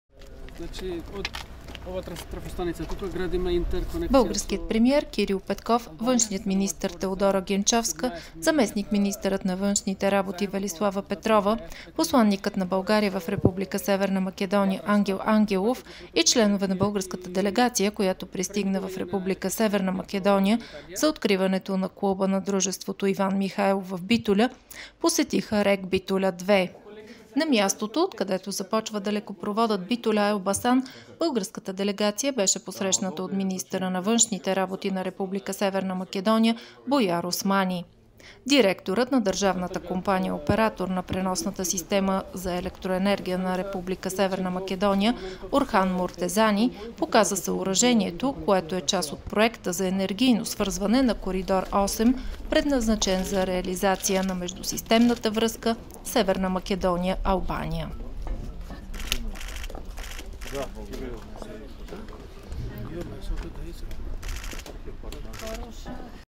Премиерът Кирил Петков посети далекопровода Битоля-Елбасан
Българският премиер Кирил Петков, външният министър Теодора Генчовска, зам.-министърът на външните работи Велислава Петрова, посланикът на България в Република Северна Македония Ангел Ангелов и членове на българската делегацията, която пристигна в Република Северна Македония за откриването на клуба на дружеството "Иван Михайлов" в Битоля, посетиха РЕК Битоля 2.
На мястото, откъдето започва далекопроводът Битоля-Елбасан, българската делегация беше посрещната от министъра на външните работи на Република Северна Македония Буяр Османи. Директорът на МЕПСО (държавната компания оператор на преносната система за електроенергия в Република Северна Македония) Орхан Муртезани показа съоръжението, което е част от проекта за енергийно свързване на Коридор 8, предназначен за реализация на междусистемната връзка Северна Македония-Албания.
"Двете правителства работят много усилено. Факт е, че често се срещаме, имаме ясни задачи, сега трябва да ги изпълним и за да напреднем. Силно сме убедени, че европейското бъдеще на Република Северна Македония е жизнено важно за стабилността на България. Така, че действаме оперативно по задачите, които сме си поставили", каза Петков в отговор на въпроса ще вдигне ли България ветото за Република Северна Македония.

Текст - Маринела Величкова
Кадри - Пресслужба на Министерски съвет